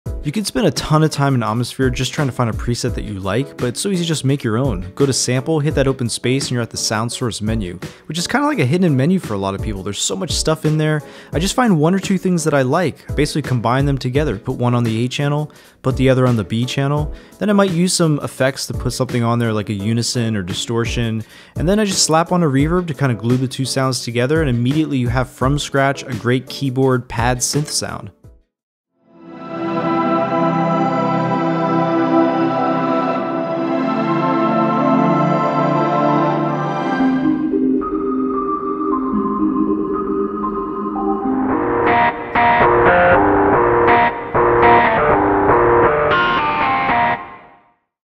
How to Make Sounds in Omnisphere
Many people don't know how to make sounds in Omnisphere when they buy it. While the preset menu is great, the Omnisphere soundsource menu allows you to stack samples together quickly and make totally original sounds from scratch very easily.